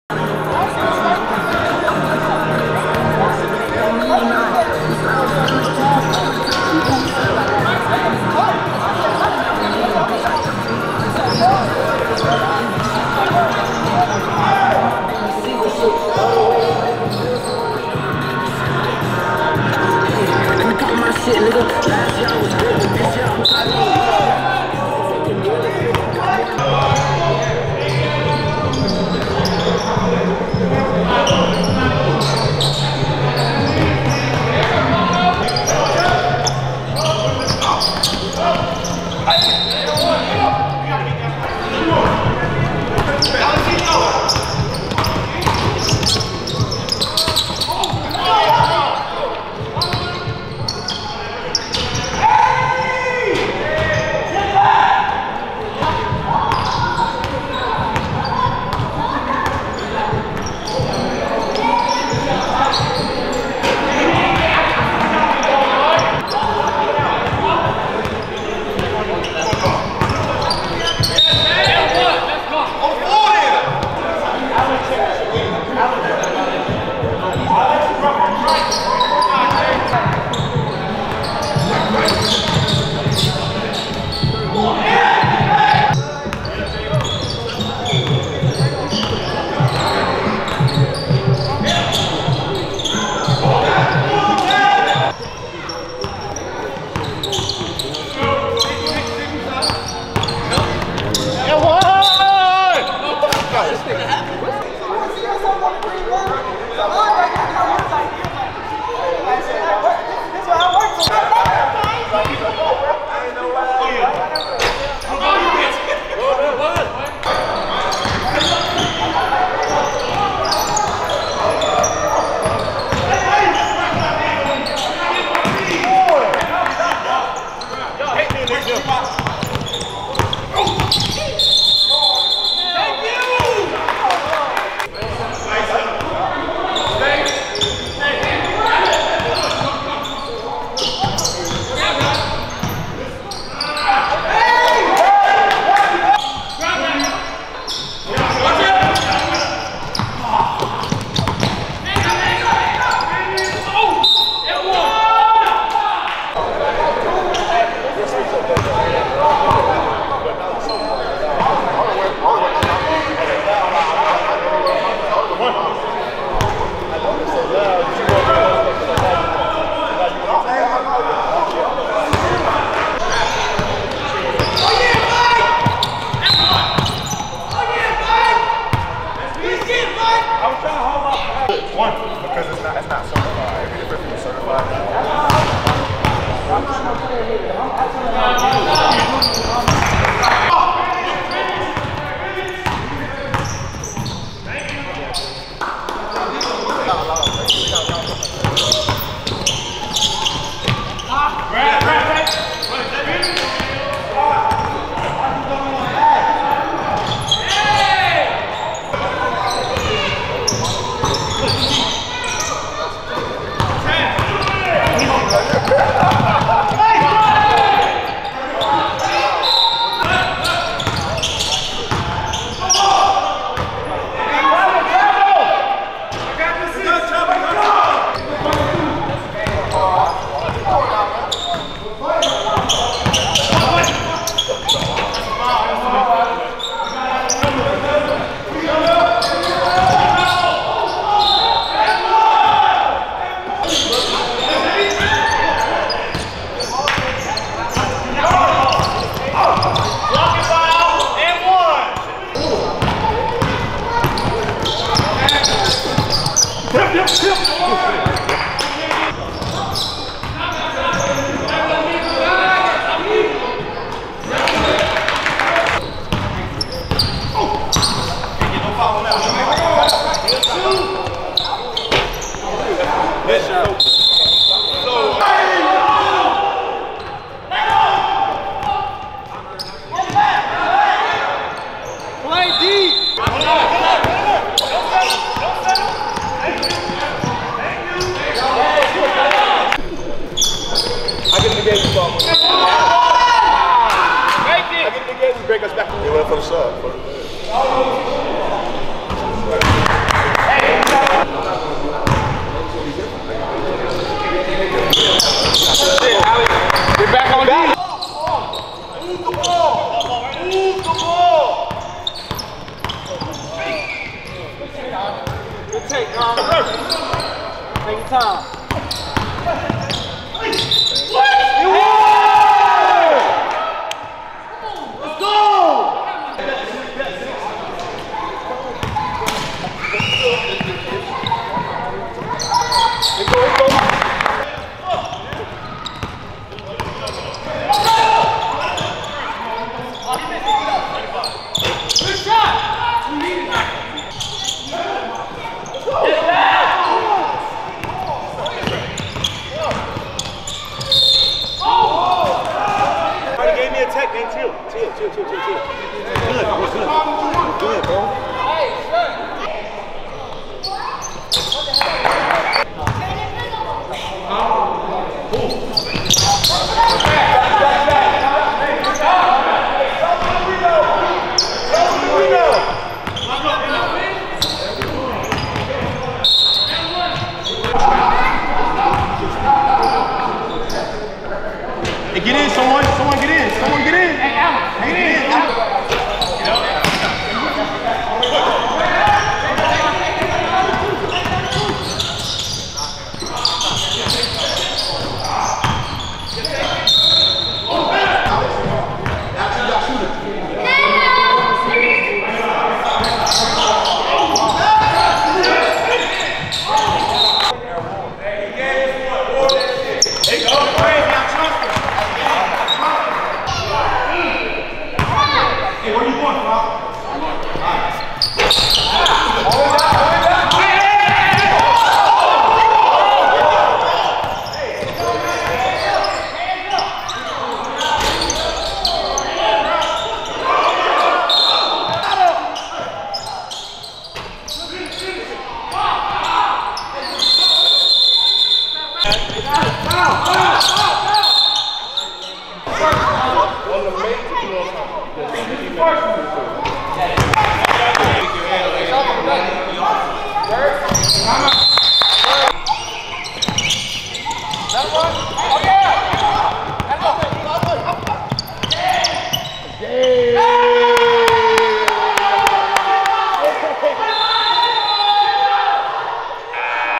VIP ELITE VS OUTLAWZ Supreme Basketball Highlights
Supreme Flag Football Presents Supreme Basketball League
Supreme Flag Football Started A mens Basketball League Here is Key Highlights From First Ever Championship Game A Division

Vip Elite (2) vs Outlawz (1)